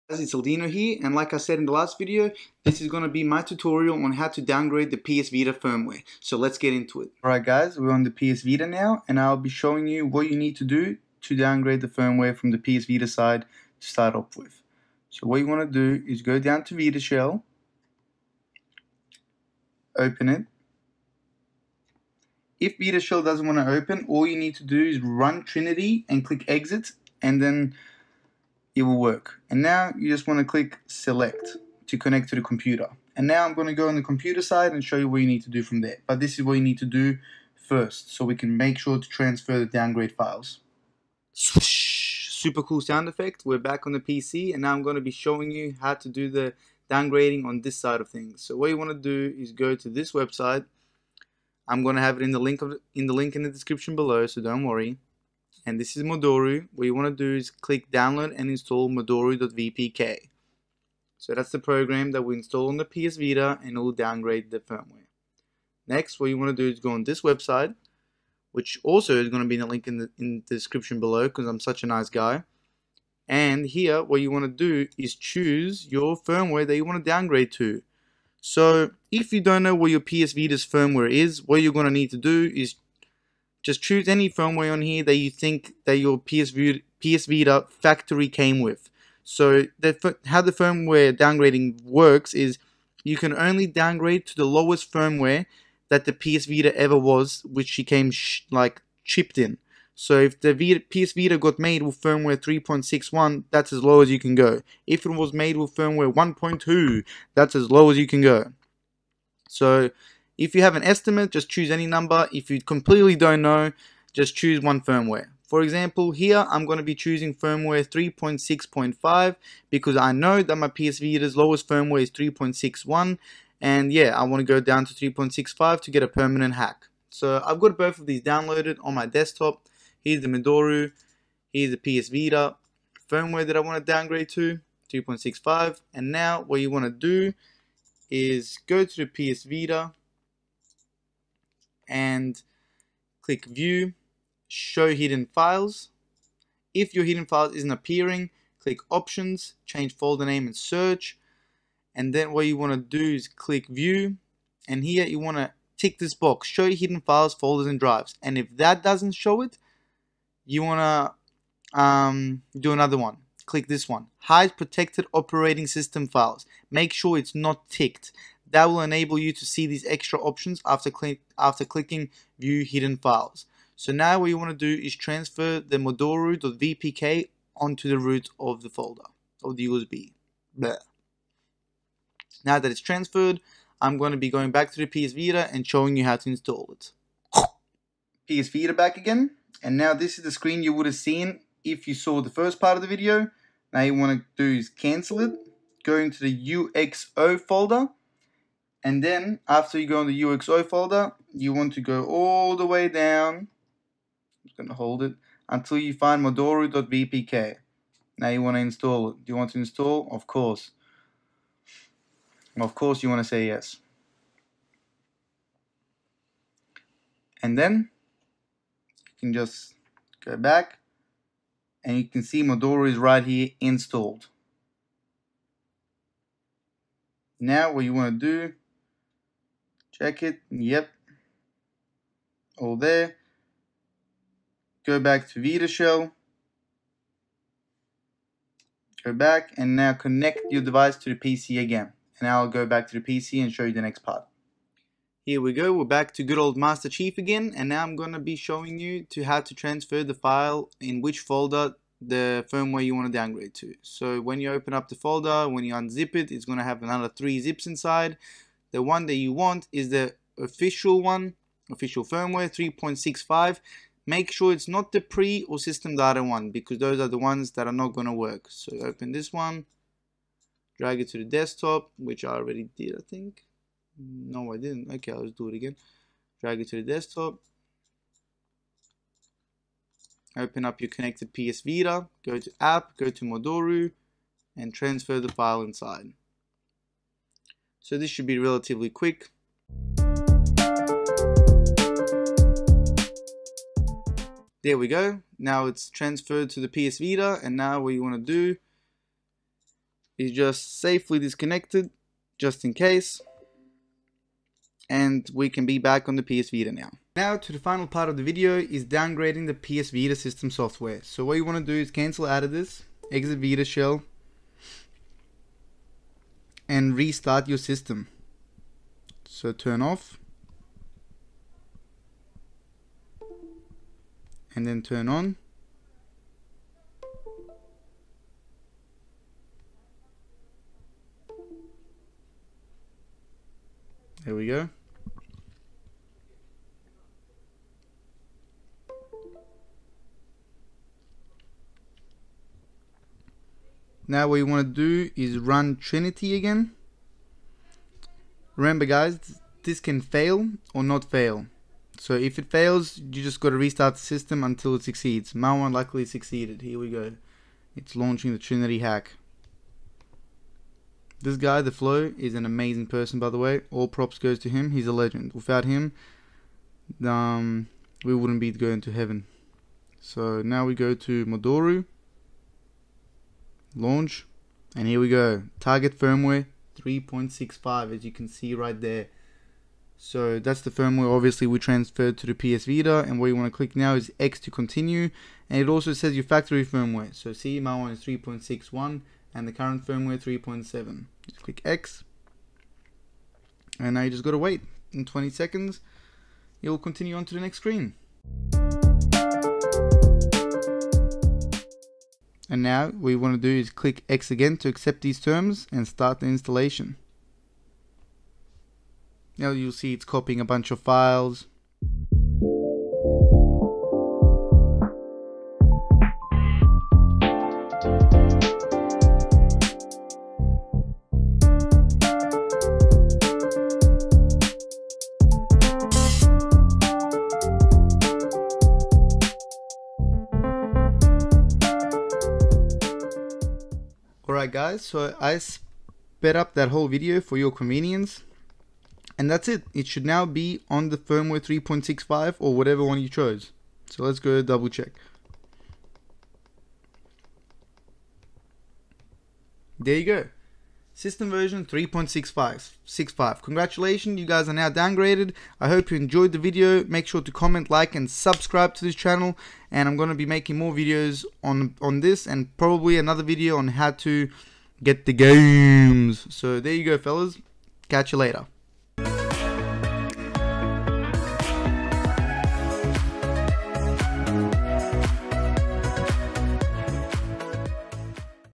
How to Quickly Downgrade Any PS Vita Firmware - Tutorial Video
How to quickly downgrade any firmware on your PS Vita. The video will be downgrading a 3.70 firmware to 3.65. However, it is the same on any other firmware.